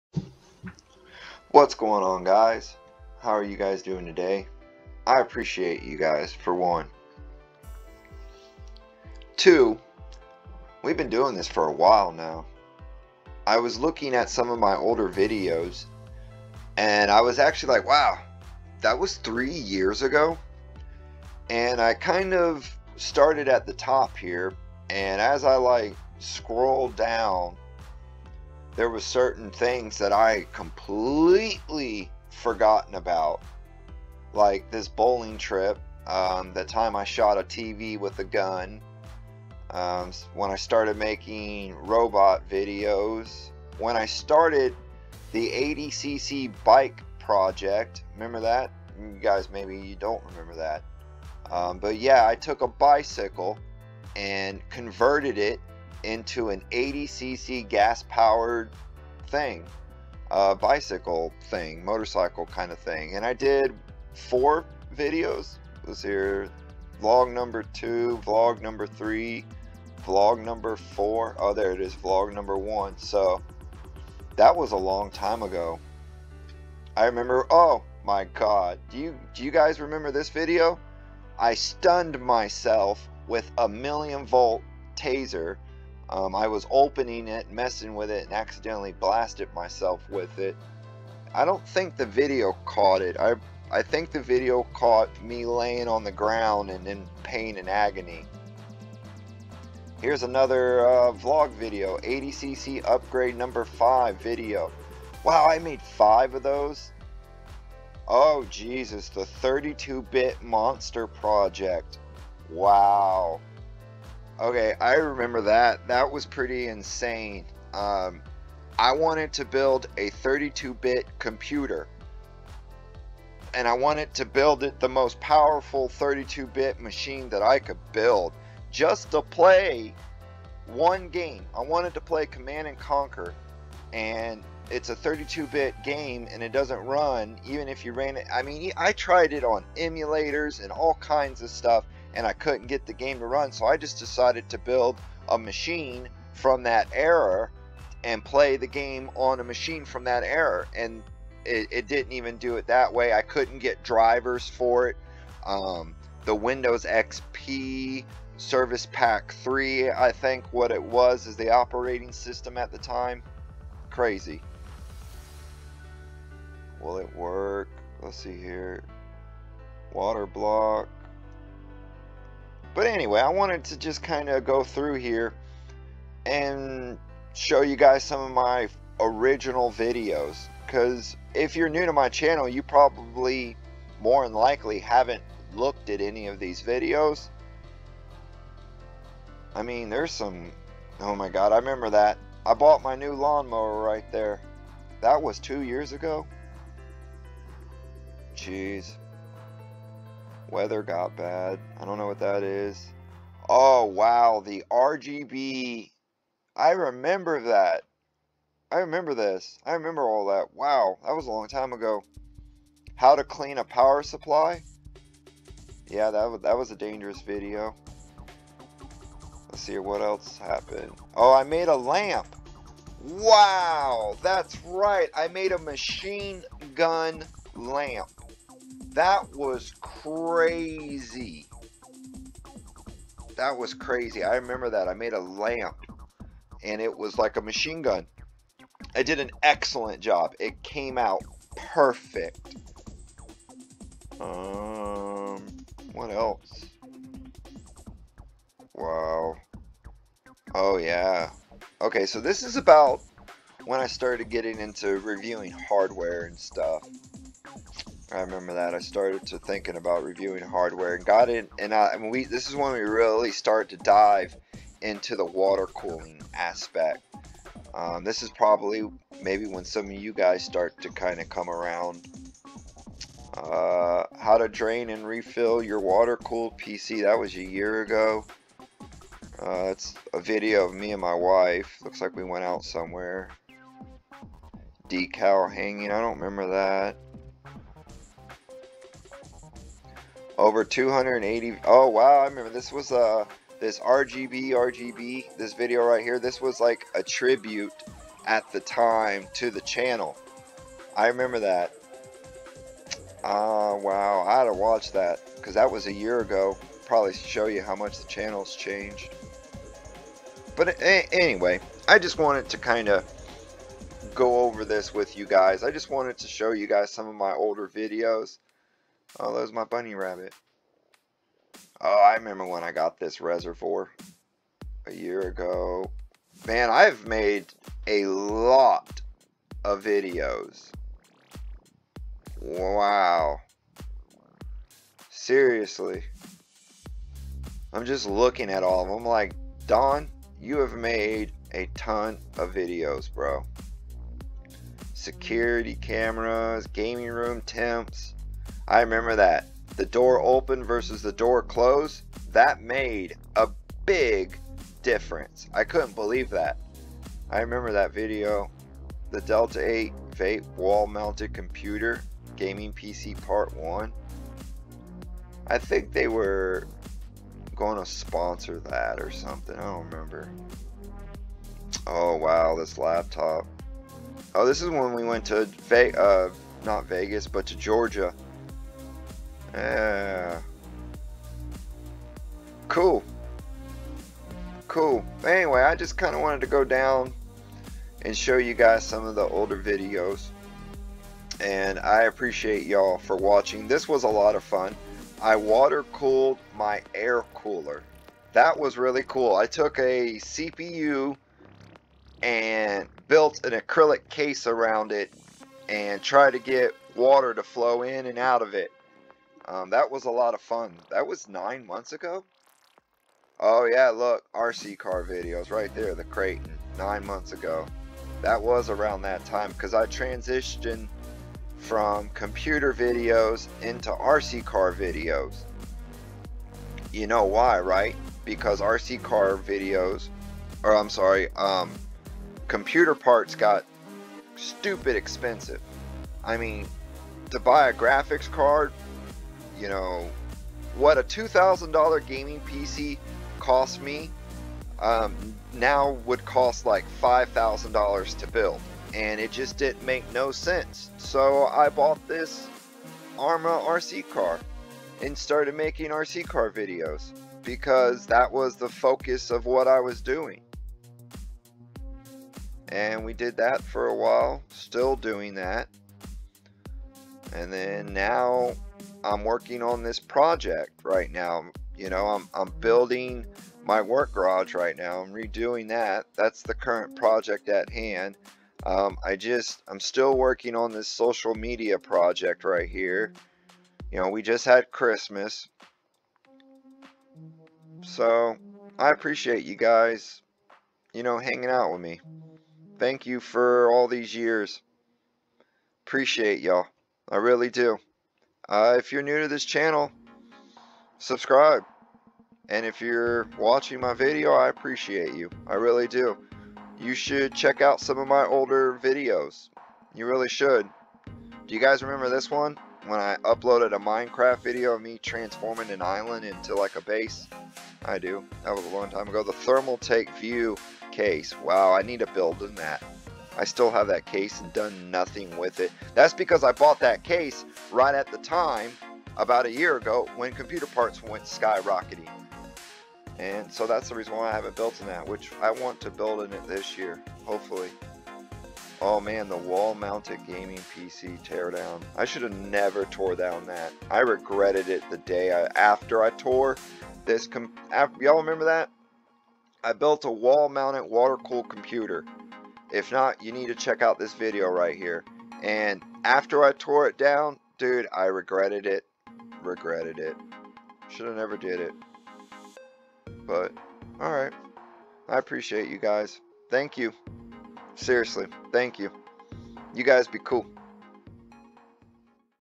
iv Made ALOT of Videos | 3 years of Making Videos on YouTube
Iv been making YouTube videos for 3 years now. i bet there are some videos of mine you have never seen before.  here's a 9min blast to the past tour of my channel.  Thank You!!
 * CHECK US OUT ON *SOCICAL MEDIA **   👇👇👇👇 $$   LINKS BELOW $$

P.O.BOX 1478
Pigeon Forge ,TN
37868                                   👇👇👇👇

$jimhamilton85